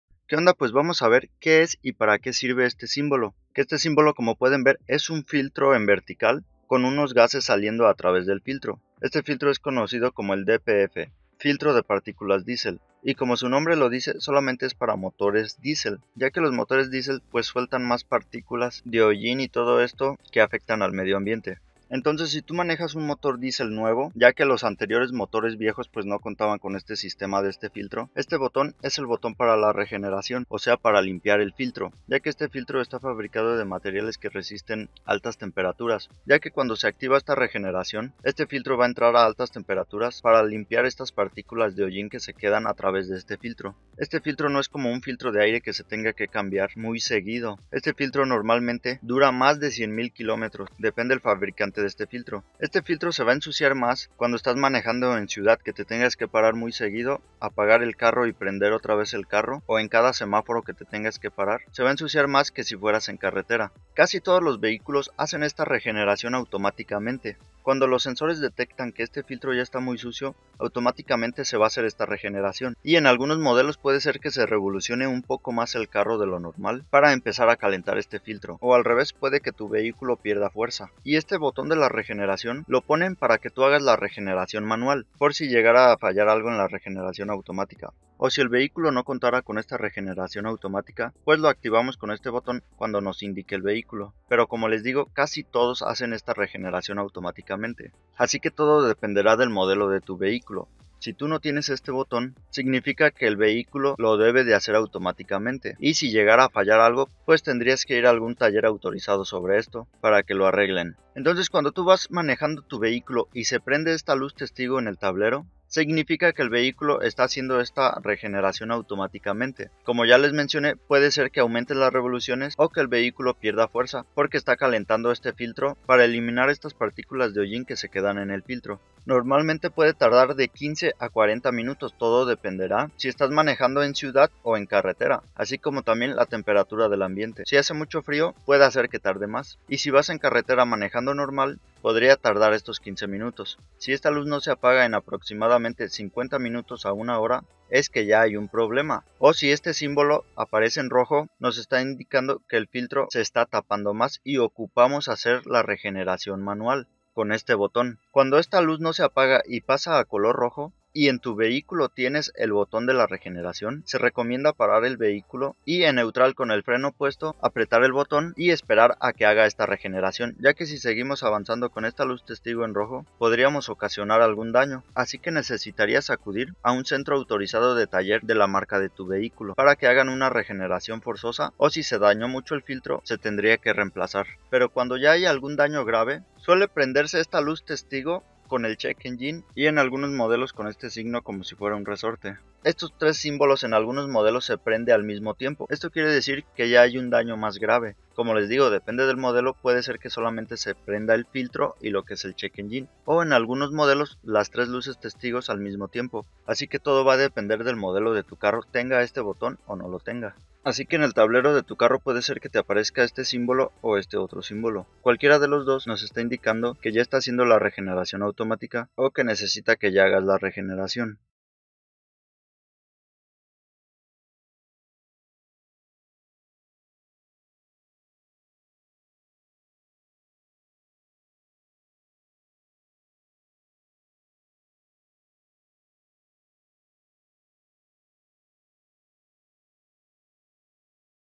¿sabes para que sirve este boton? - filtro DPF (filtro de particulas diesel)
como funciona el filtro de particulas diesel y boton de la regeneracion, algunos carros diesel nuevos tiene este boton y esta luz testigo del DPF se enciende y nos avisa que el vehiculo necesita regenerar el filtro de particulas.